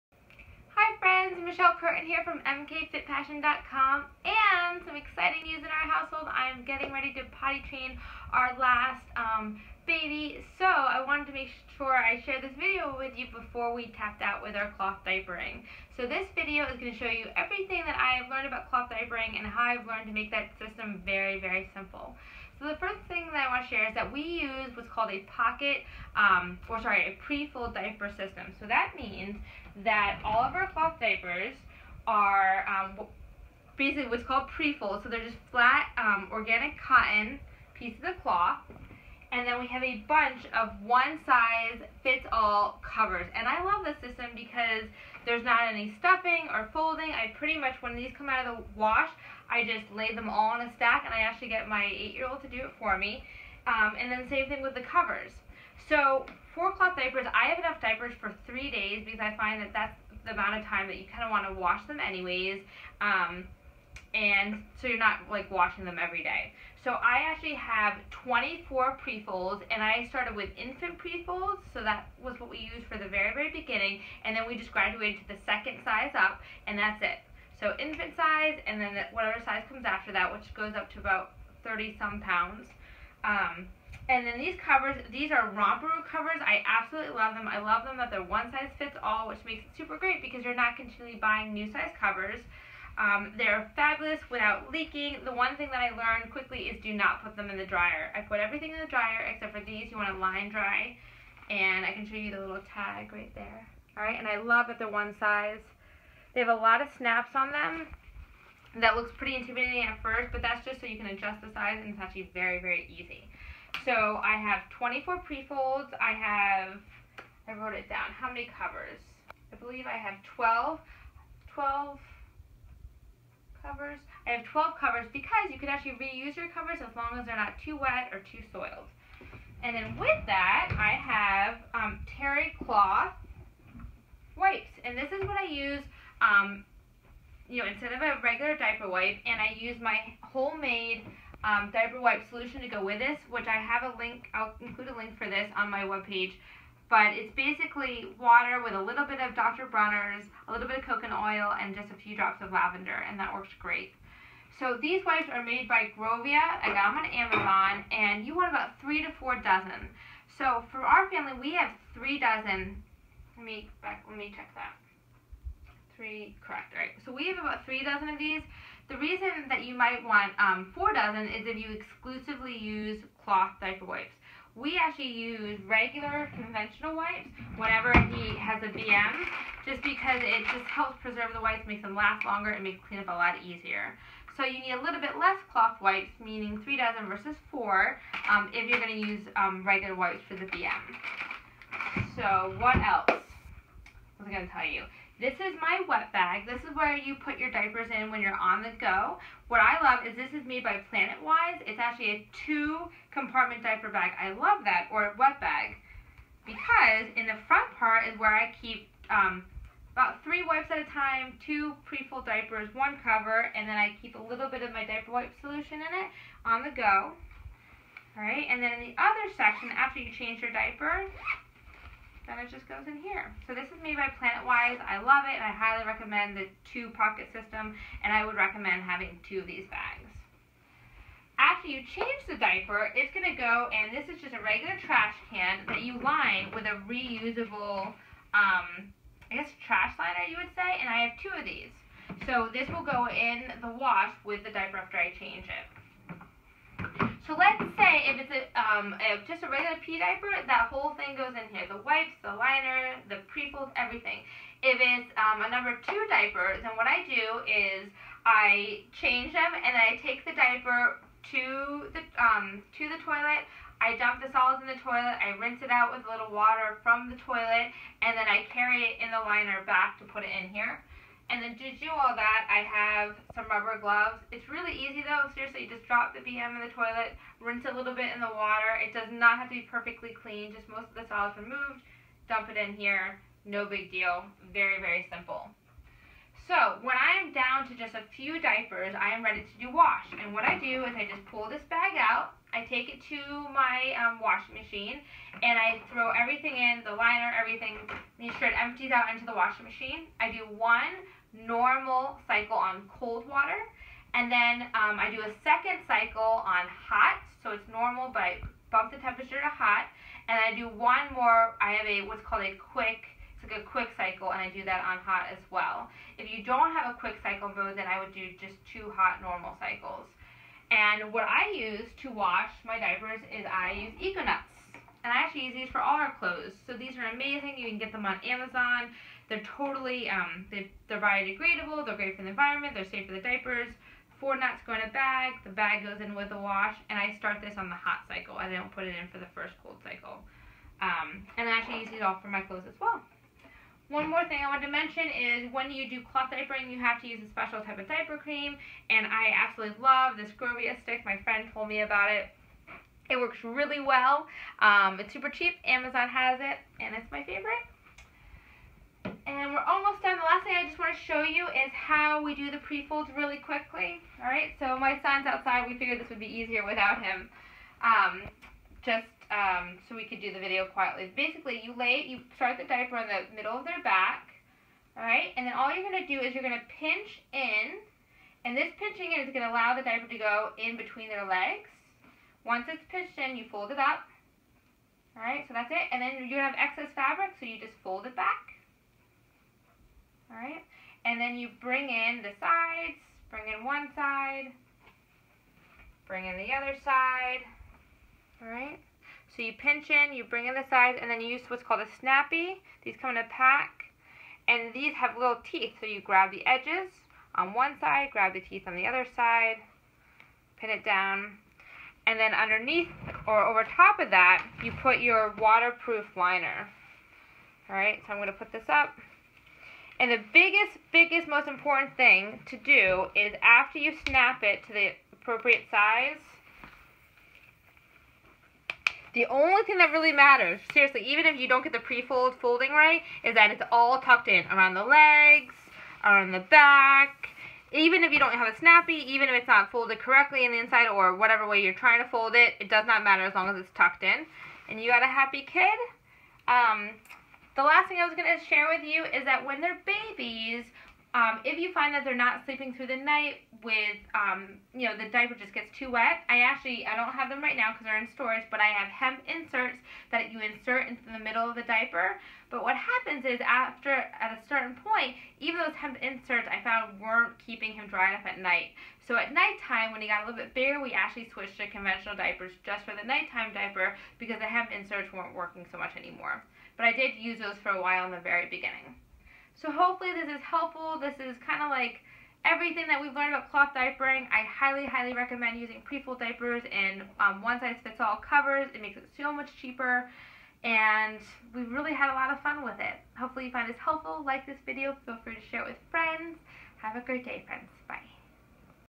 How To Cloth Diaper With Ease!
This video will show you all of my tips and strategies to make cloth diapering super easy.  There are so so many ways to cloth diaper and I really break down all of the options I chose and how they helped to simplify the process.

Love,
Michelle